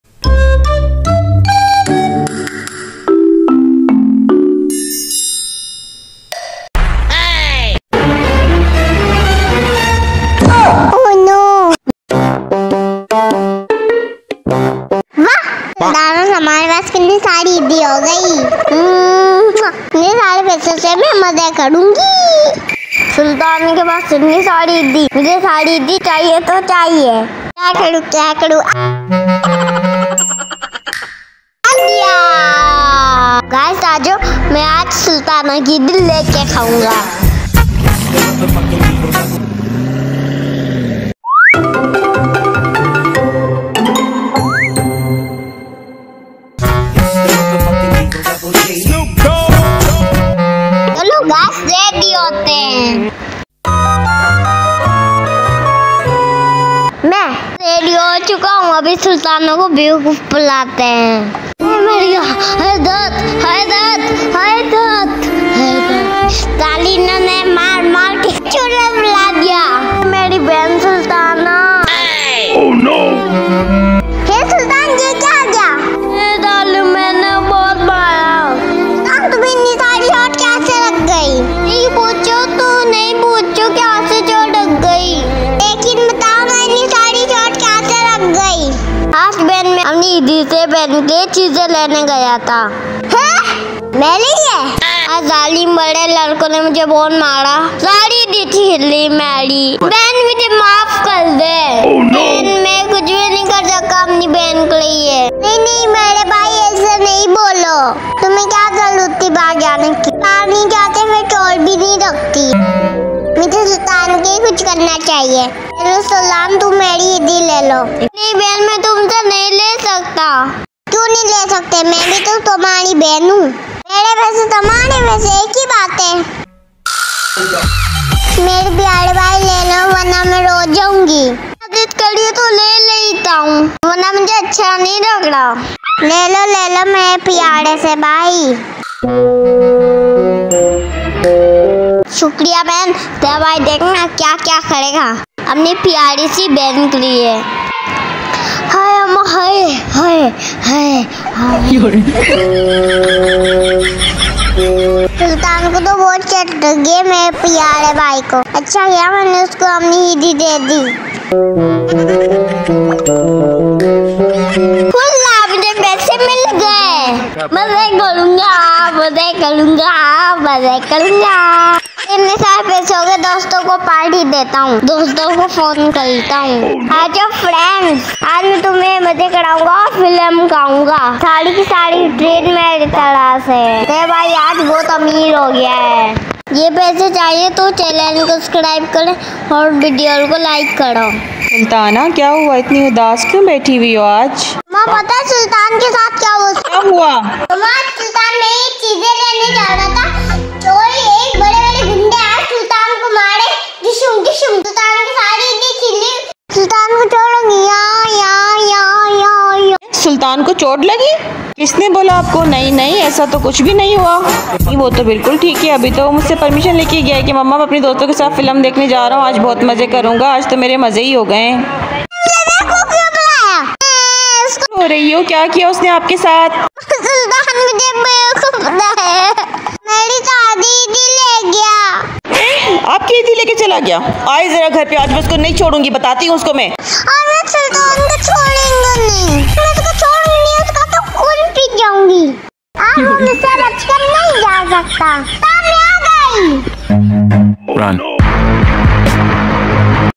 EIDI| Siblings| Short Film
This is an entertaining story of How cleverly sultan made plan to get Sultana's EIDI. Hopefully with our litter effort we make enabled our self to bring smile onto your face in the world of worries.